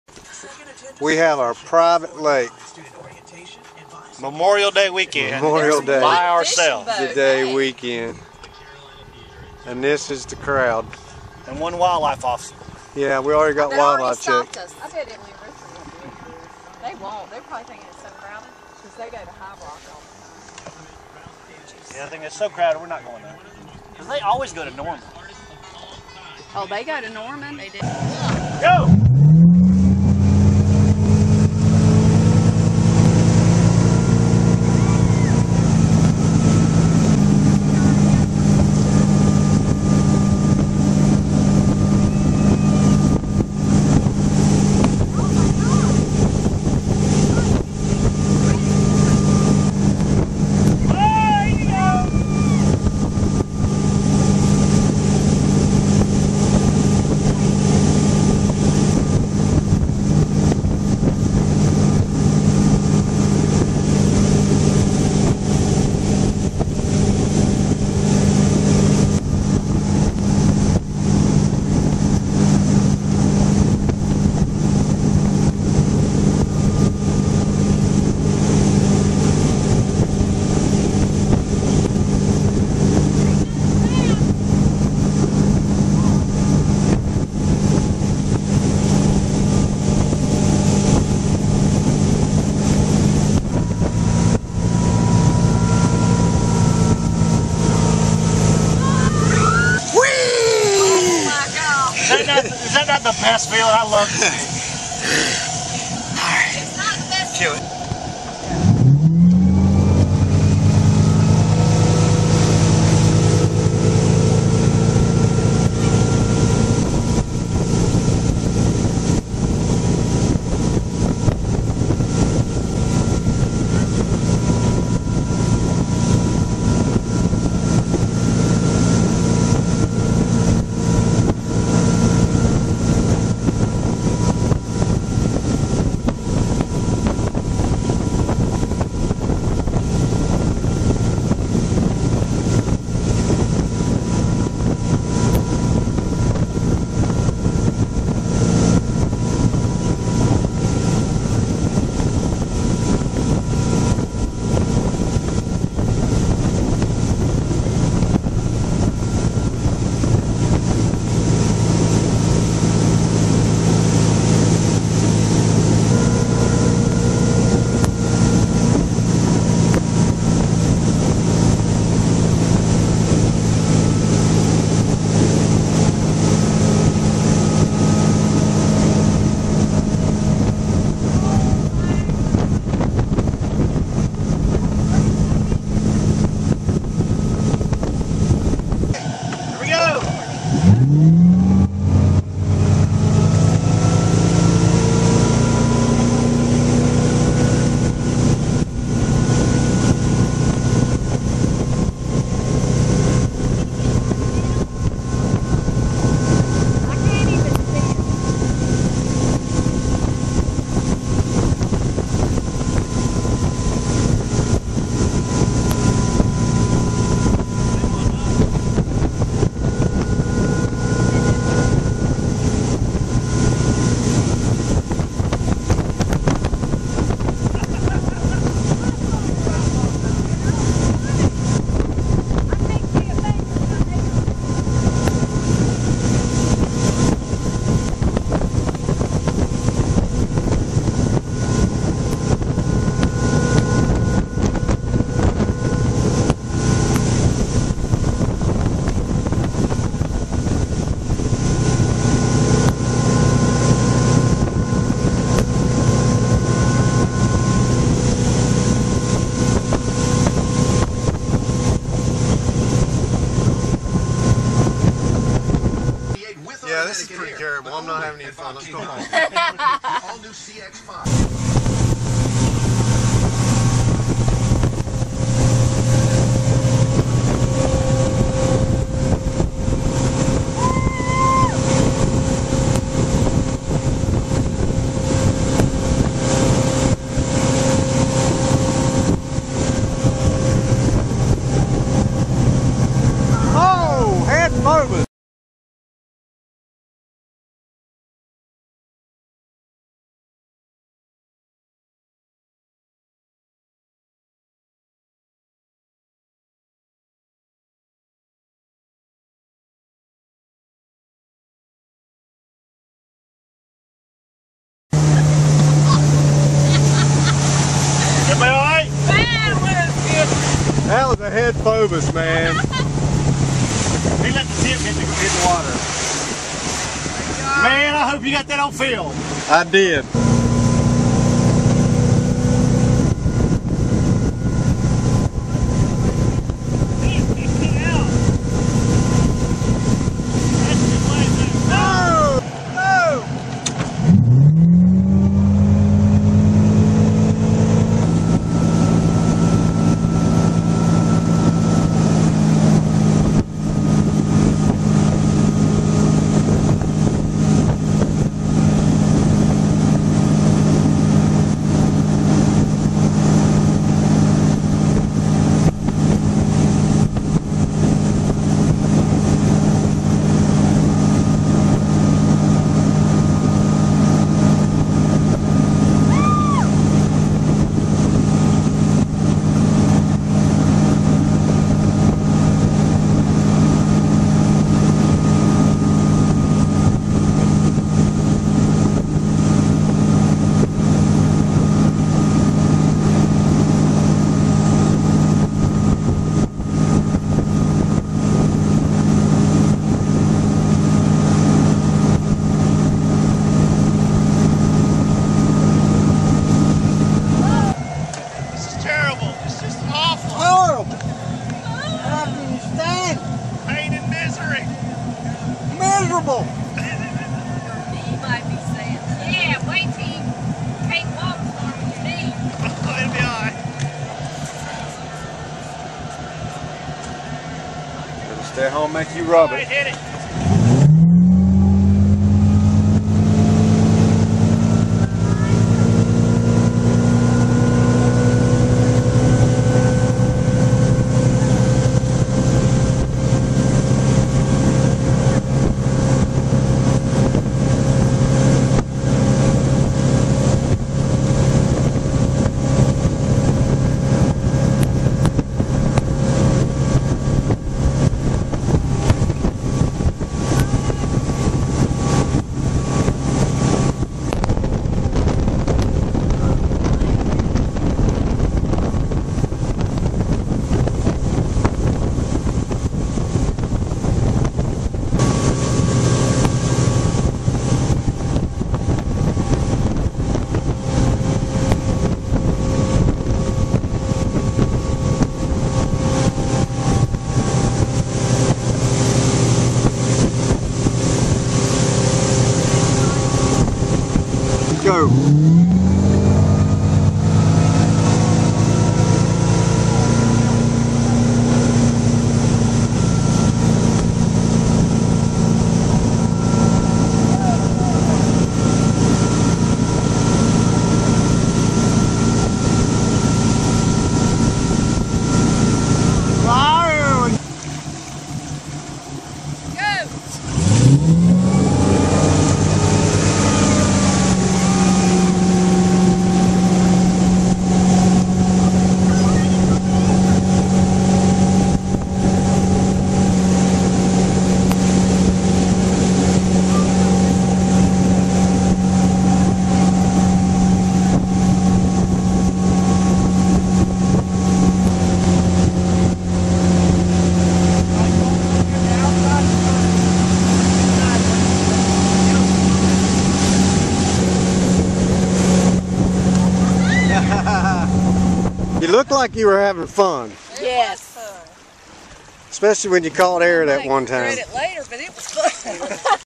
Oak Hollow Water Skiing 2012.mp4
Memeorial Day with the In-laws.  What a Great Weekend!